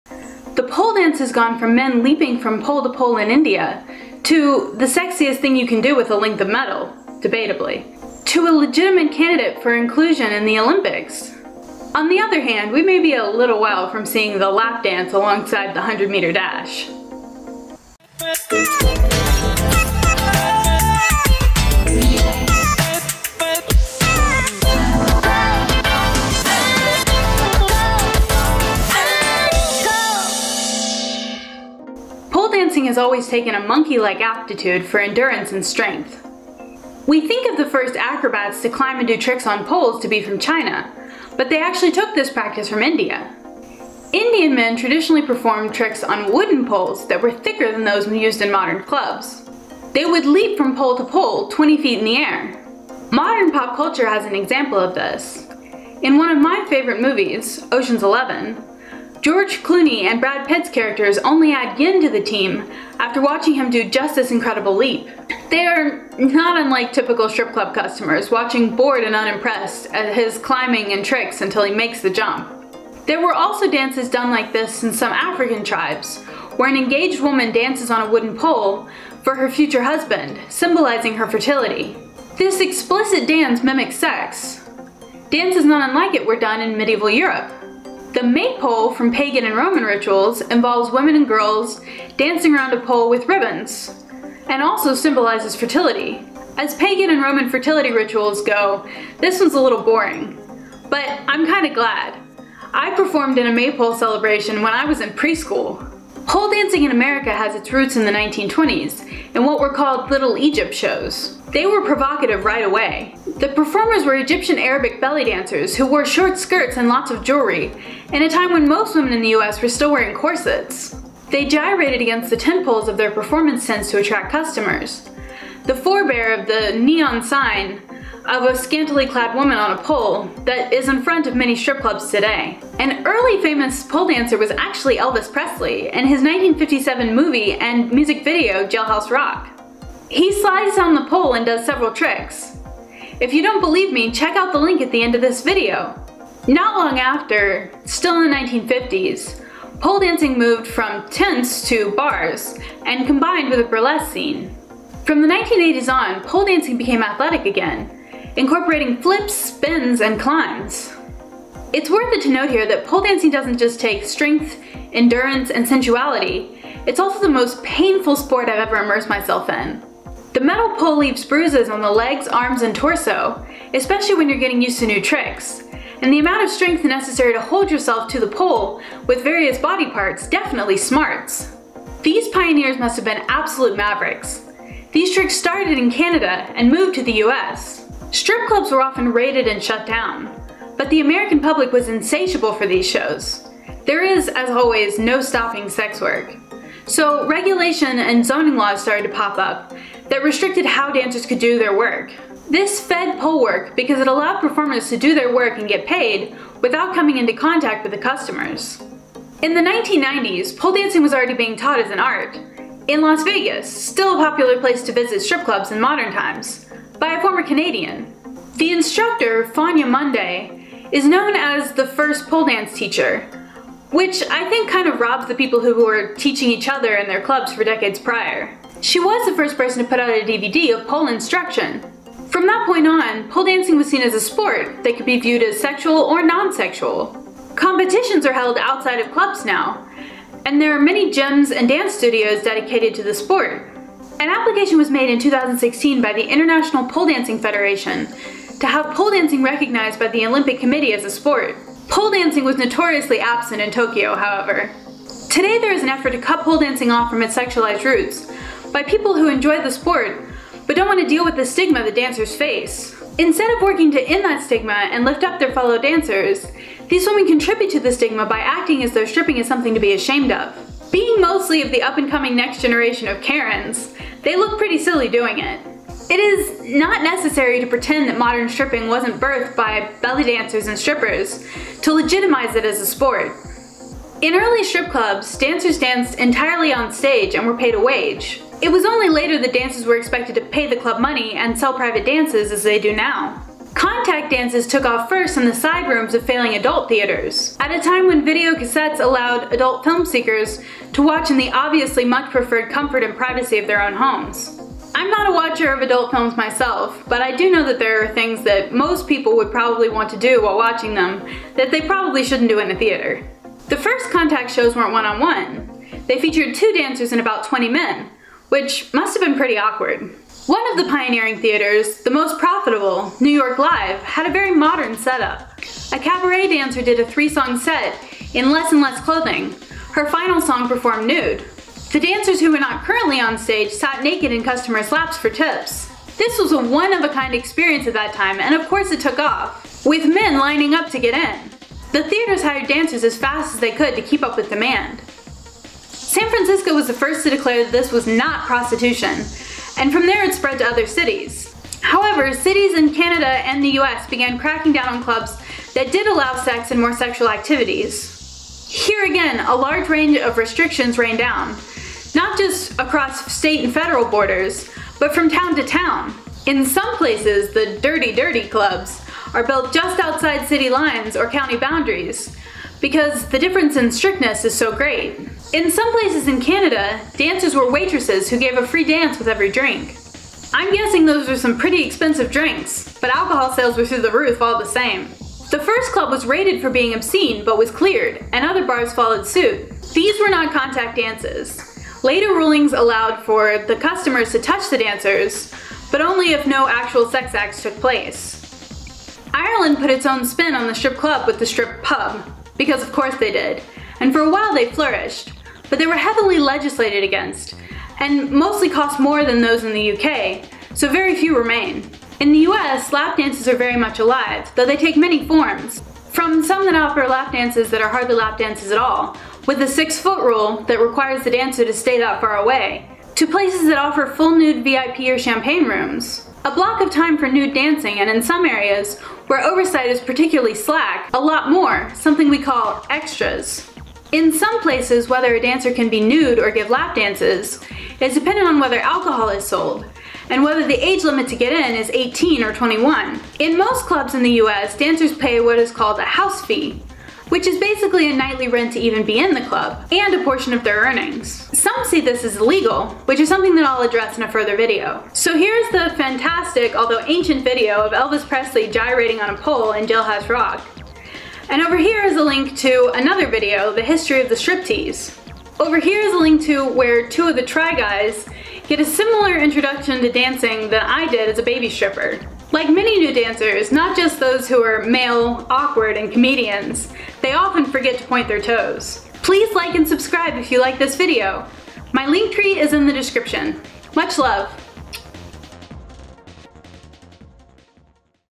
The History of Lap Dancing and Pole Dancing!
The very ancient story of the pole dance up to modern times, and the much less complicated story of the lap dance.

Doing acrobatics on poles started in ancient India, performed by men, and spread to the rest of mainland Asia. Dancing on and around poles for fertility or sexy reasons popped up in many places, on many time lines, from ancient Pagan and Roman May Pole celebrations to African engagement celebrations, to belly dancers in 1920s America dancing on tent poles in traveling shows.

Modern pole dancing evolved from that tent pole dancing and was spurred by laws that required dancers to stay away from customers as they stripped or when they were naked.

Lap dances have much less deep roots, although sitting in someone's lap as a flirtation technique is probably as old as humanity. Contact dances came about in failing adult film theaters looking for something to fill the gap as people began watching their adult films on home VCRs. These had a rocky time with the law and are very different today from one country or state to another and even one county to another.